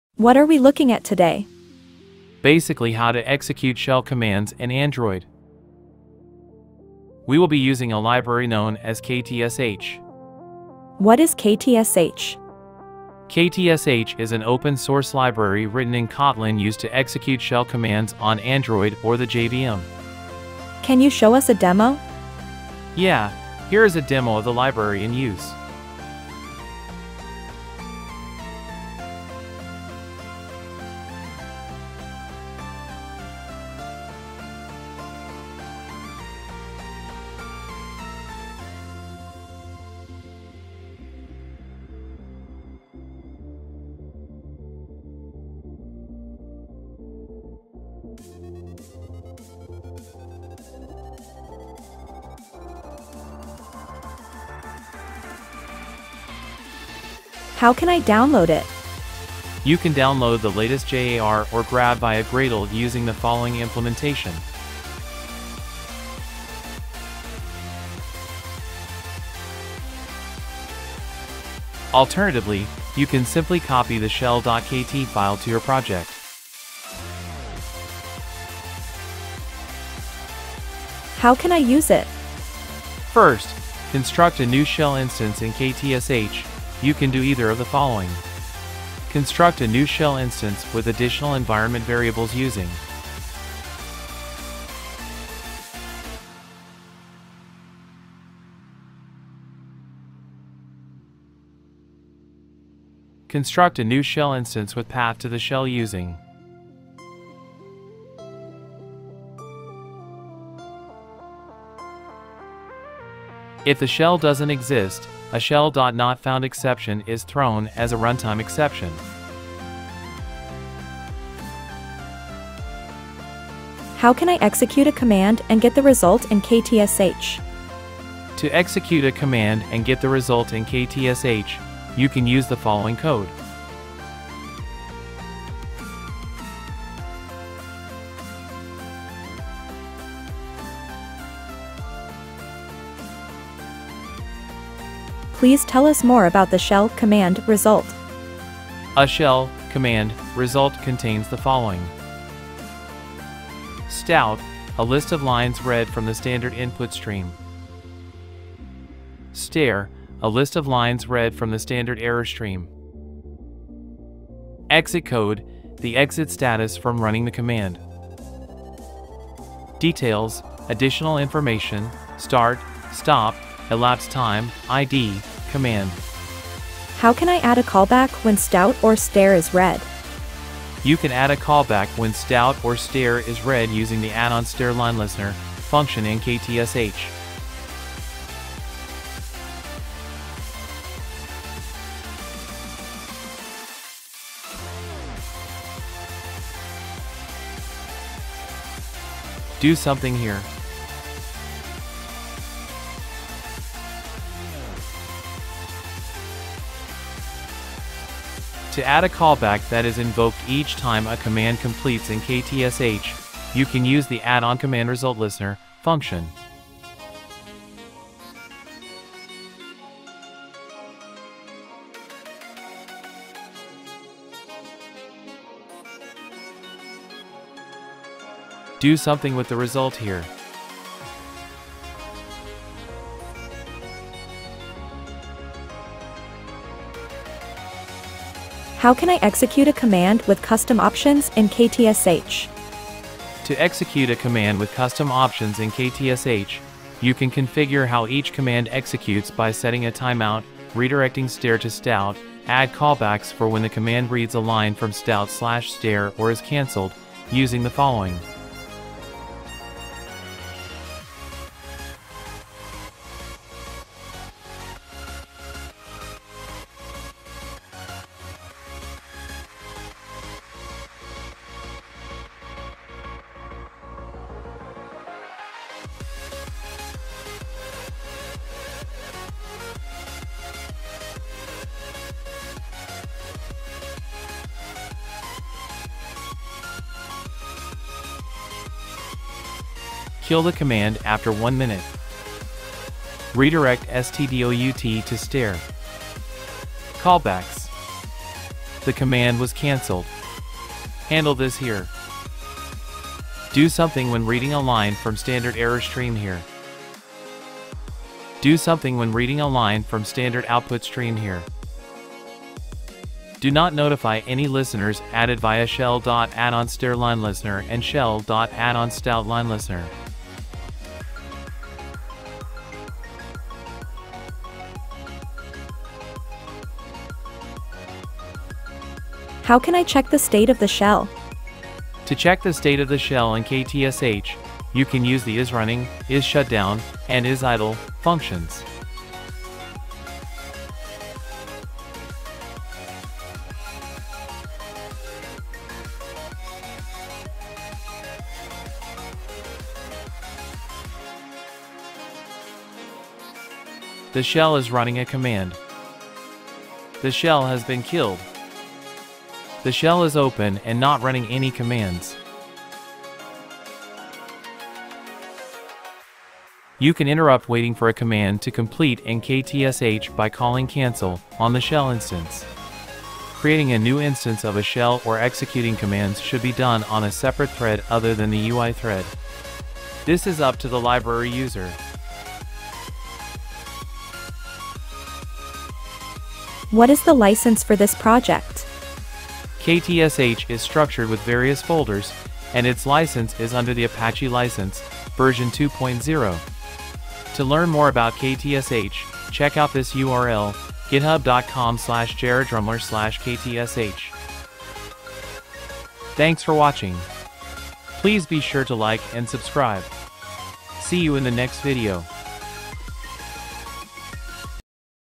How to Run Shell Commands in Android
Install the app and access over thousands of awesome libraries and how to use them for free.

How to execute shell commands in android. We will be using a library known as KTSH.

KtSh is an open source library written in Kotlin used to execute shell commands on Android or the JVM.